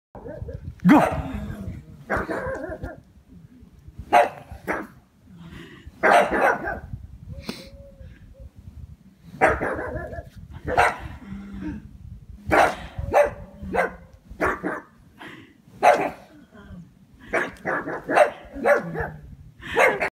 PUGS | And Scary Woman
Watch the team of pugs met the scary woman!